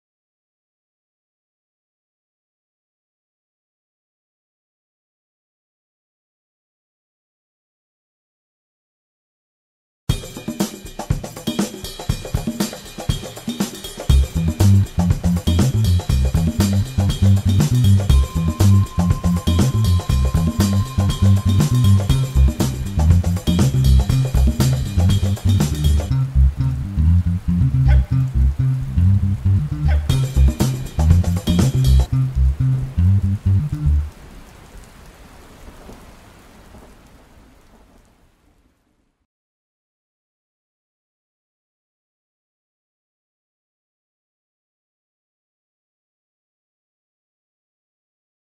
Purple faced langurs (Semnopithecus vetulus nestor)
Footage of the Semnopithecus vetulus nestor or Purple faced Langur in Athurugiriya near Colombo, Sri Lanka - December 2011. This species is considered extinct in this region.